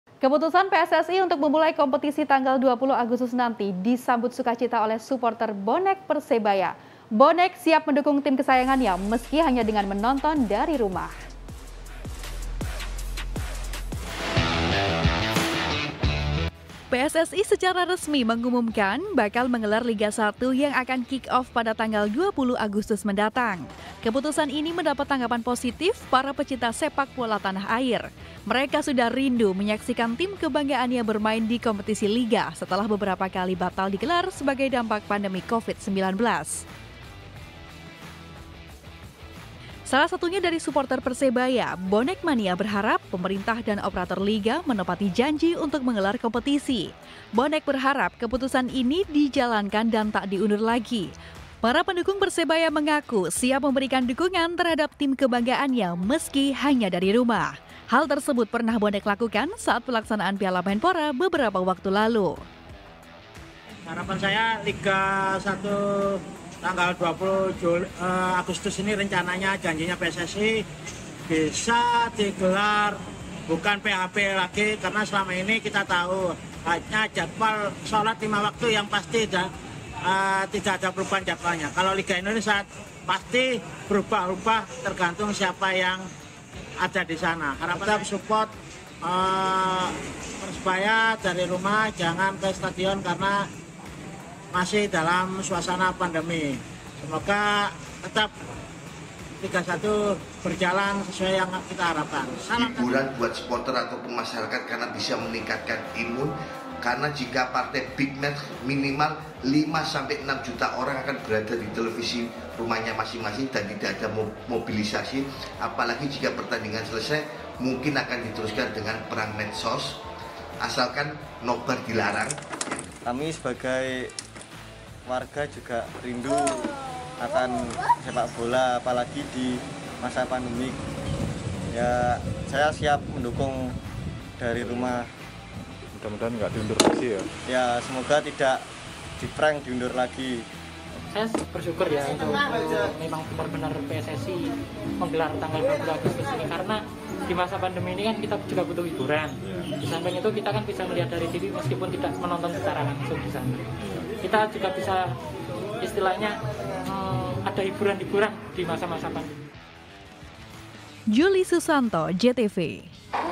Bonek Siap Dukung Persebaya dari Rumah
Keputusan pssi untuk memulai kompetisi tanggal 20 agustus nanti disambut sukacita oleh suporter bonek persebaya. Bonek siap mendukung tim kesayangannya meski hanya dari rumah.